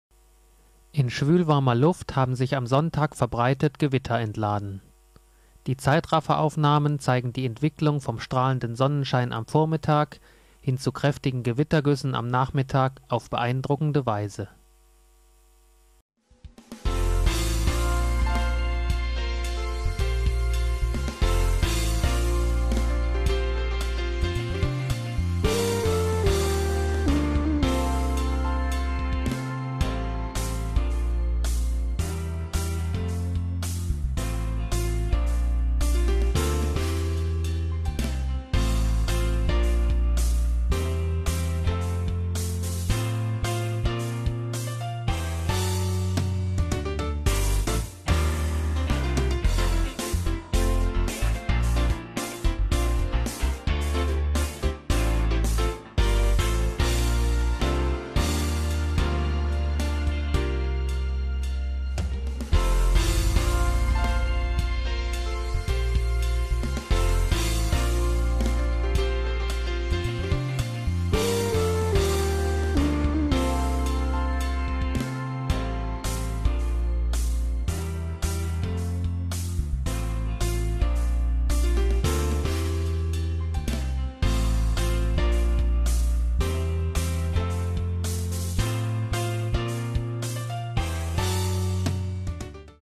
Gewittergüsse in Hessen - Wolkenrotation und Regenvorhänge
Beeindruckende Aufnahmen eines Gewittertages. Nach zunächst noch strahlendem Sonnenschein entwickeln sich im Verlauf immer mehr Quellwolken, die sich zu Gewittergüssen ausbauen. Die Zeitrafferaufnahmen aus dem Kinzigtal bei Gelnhausen stammen von Frank Friedemann.